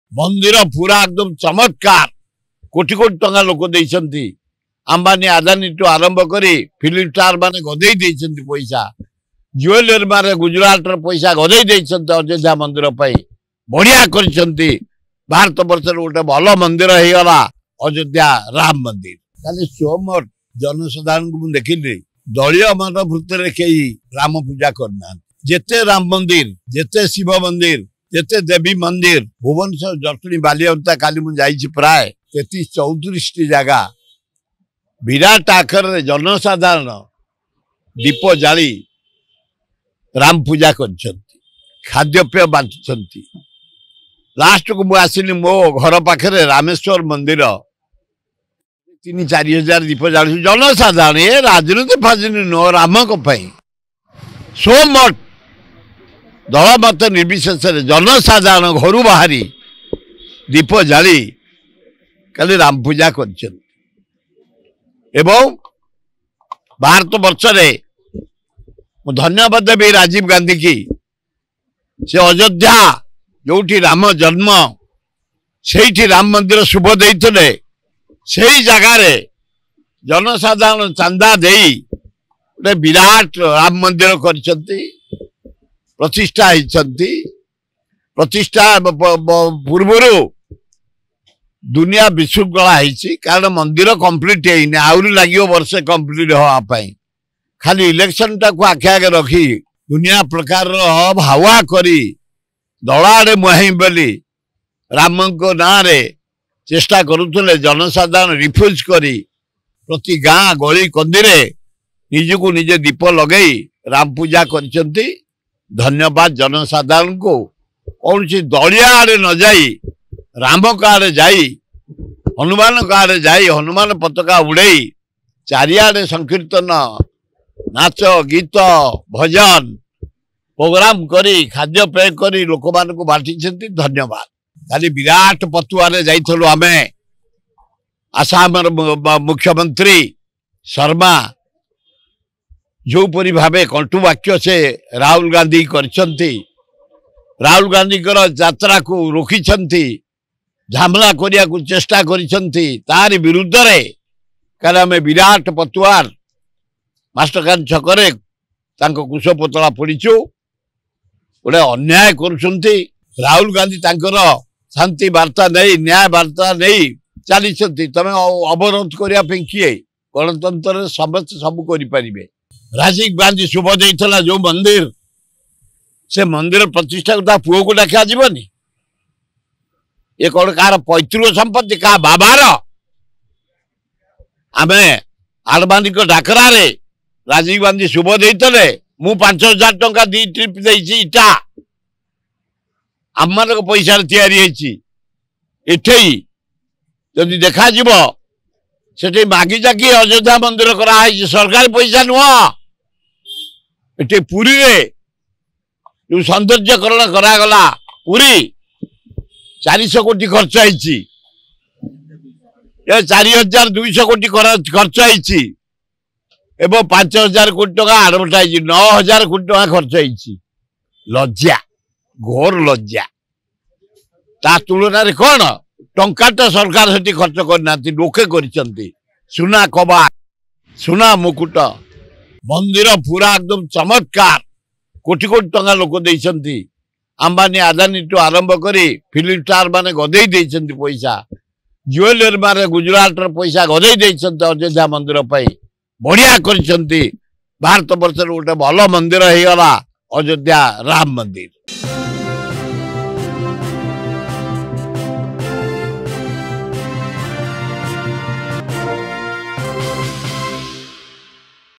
ରାମ ମନ୍ଦିର ପାଇଁ କୋଟି କୋଟି ଲୋକ ଟଙ୍କା ଦେଇଛନ୍ତି, ଫିଲ୍ମ ଷ୍ଟାର ମାନେ ଟଙ୍କା ଗଦେଇ ଦେଇଛନ୍ତି: ସୁର ରାଉତରାୟ
ରାମ ମନ୍ଦିର ପାଇଁ କୋଟି କୋଟି ଲୋକ ଟଙ୍କା ଦେଇଛନ୍ତି, ଫିଲ୍ମ ଷ୍ଟାର ମାନେ ଟଙ୍କା ଗଦେଇ ଦେଇଛନ୍ତି: ସୁର ରାଉତରାୟ, ବରିଷ୍ଠ କଂଗ୍ରେସ ନେତା